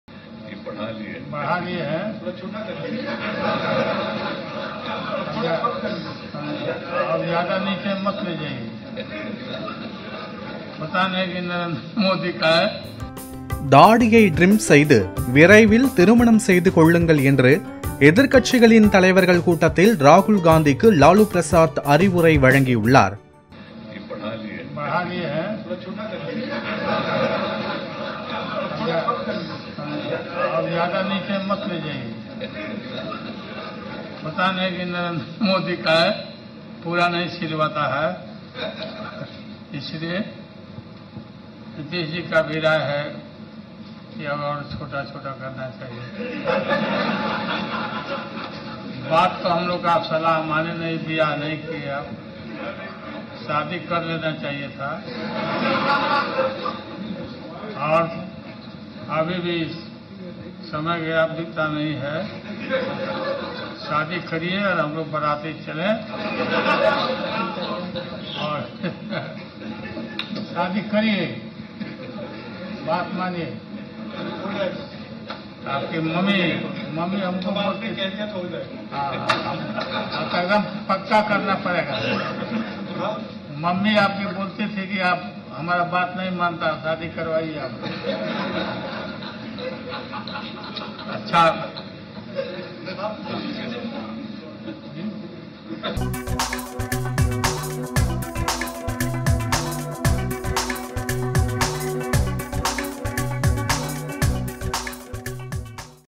தாடியை டிரிம் செய்யுங்கள்... திருமணமும் செய்யுங்கள்... லாலு அட்வைஸ் சொன்னது யாருக்கு?? | PolimerNews
தாடியை டிரிம் செய்யுங்கள்... திருமணமும் செய்யுங்கள்... லாலு அட்வைஸ் சொன்னது யாருக்கு?? | Lalu speech | Polimer News